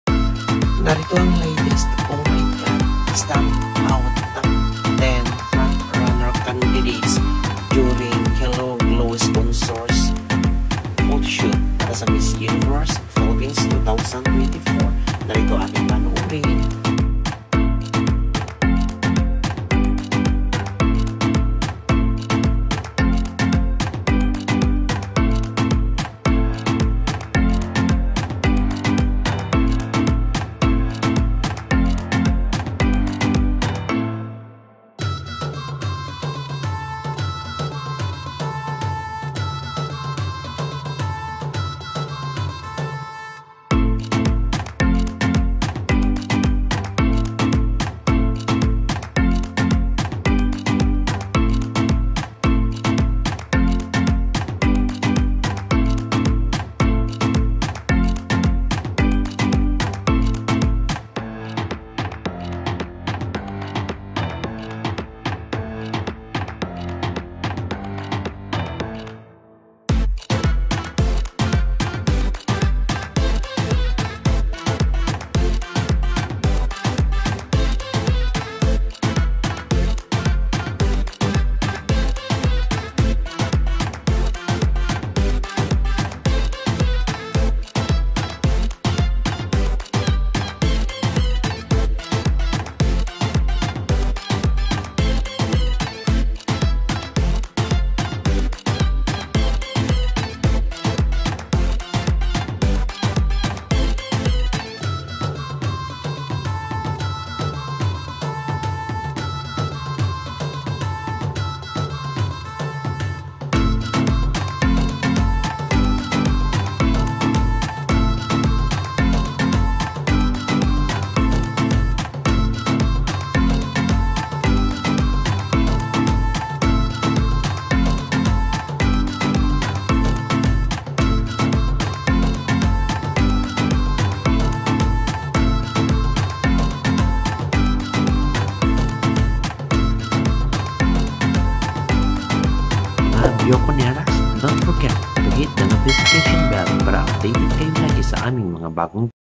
WOW! TOP 10 STANDOUT CANDIDATES DURING SPONSORS PHOTOSHOOT MISS UNIVERSE PHILIPPINES 2024
Ayan Na Top Ten Standout Candidates During Hello Glow Sponsors Shoot Miss Universe Philippines